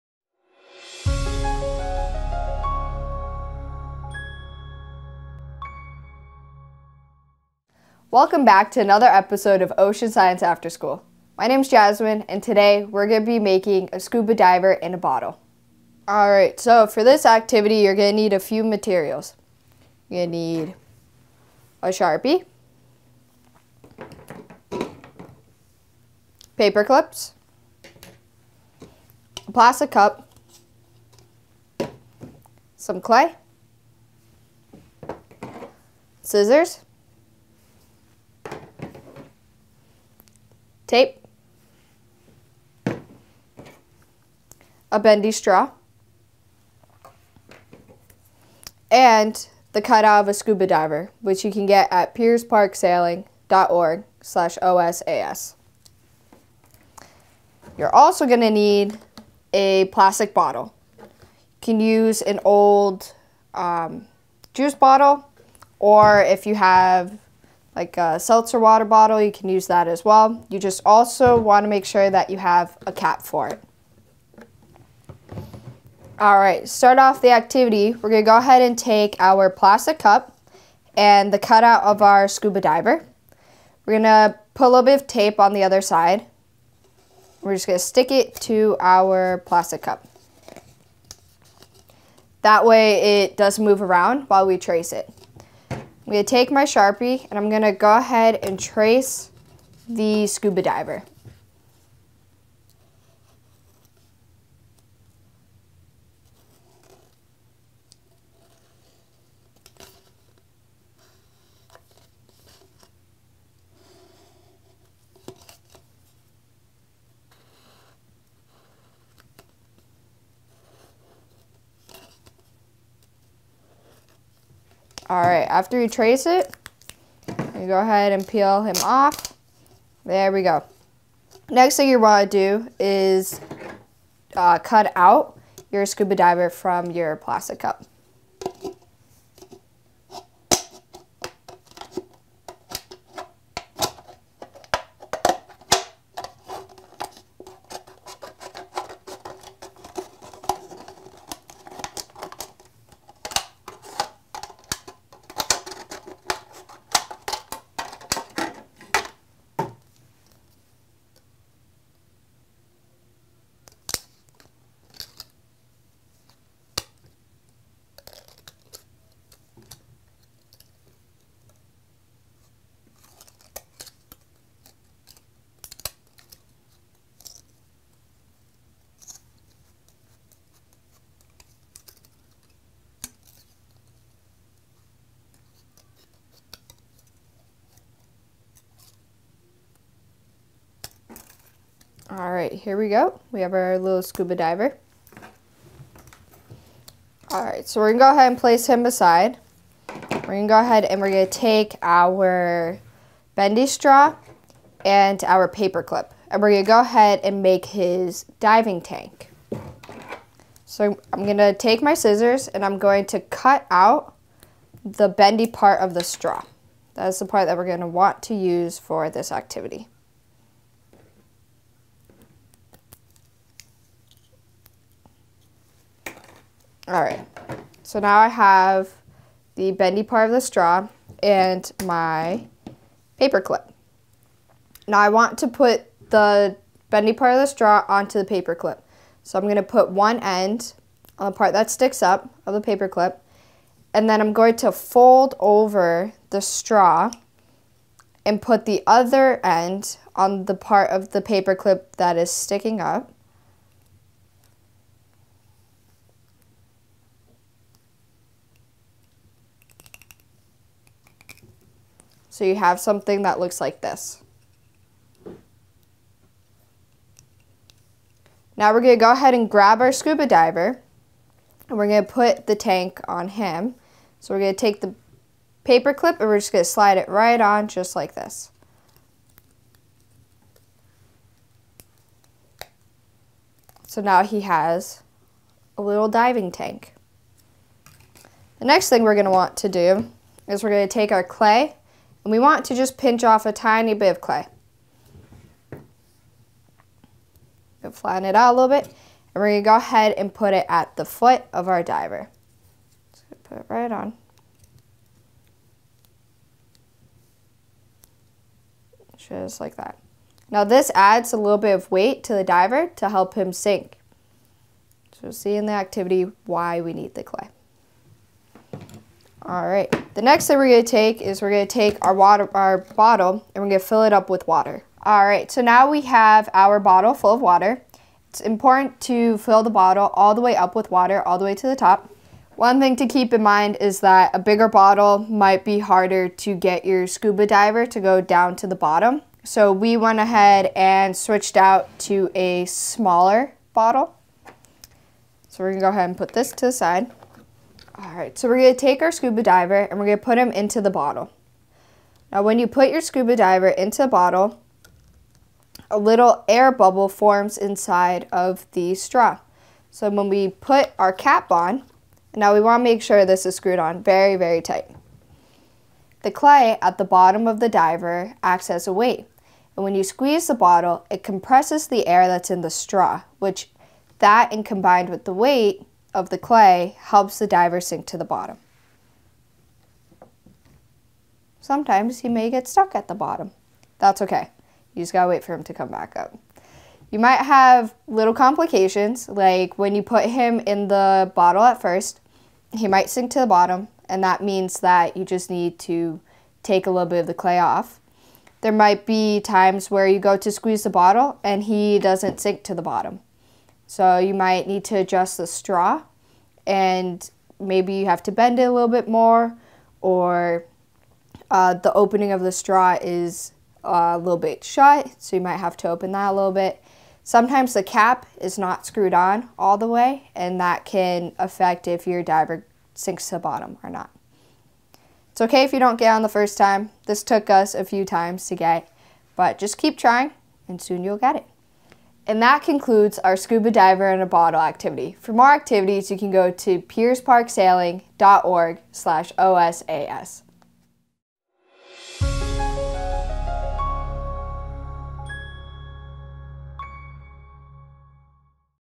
Cartesian Diver Scuba in a Bottle Activity | Ocean Science After School | Piers Park Sailing Center
Design and build a scuba diver in a bottle!  Use the principle of buoyancy and the ideal gas law to make the diver sink to the bottom and rise back up to the top.